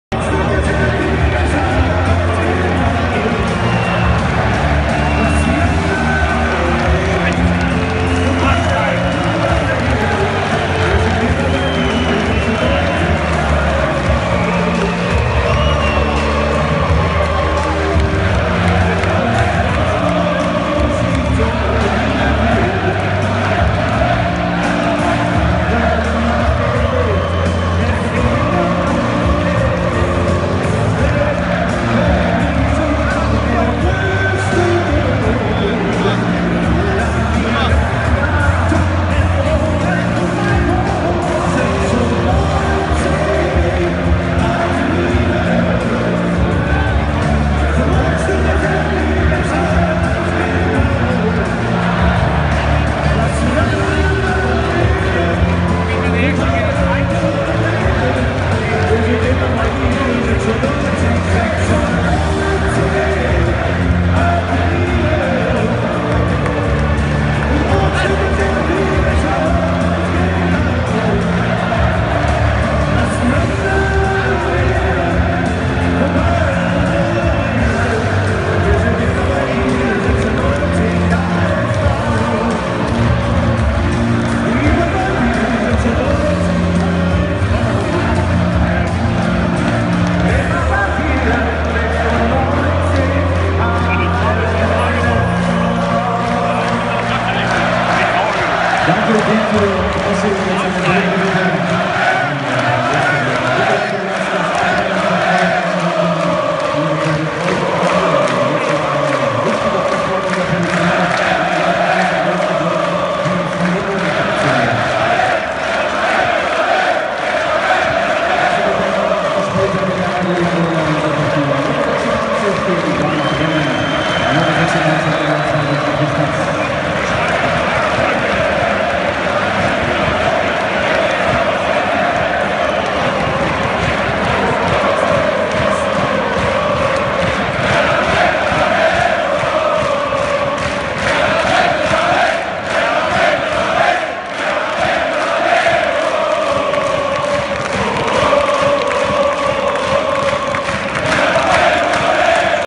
Hannover 96 - Werder Bremen 2015/2016 Pyroshow Werderfans
Pyroshow der Werderfans beim Auswärtsspiel in Hannover
2015/2016